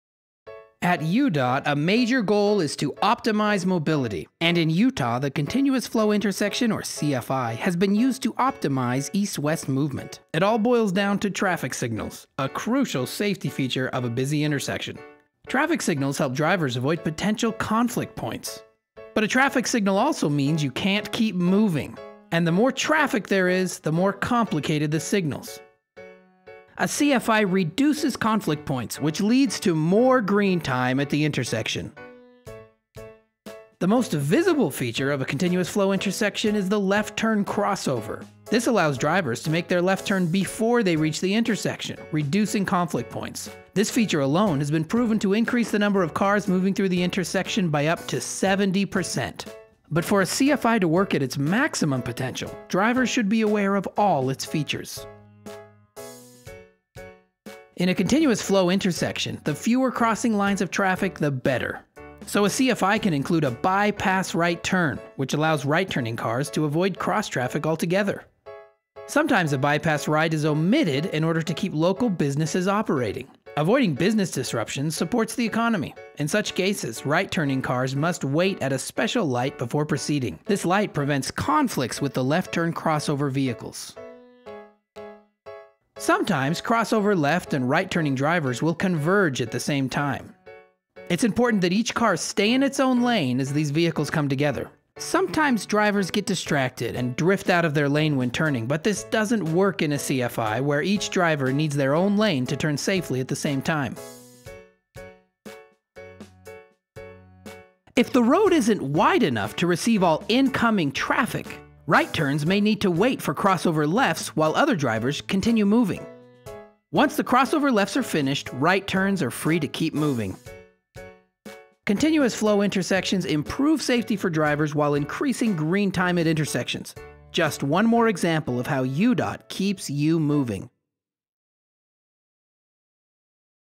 All About: Continuous Flow Intersections
2012
Client: Utah Department of Transportation
Purpose: Explain the benefits (and various configurations) of Continuous Flow Intersections (CFI) that were being implemented throughout Utah.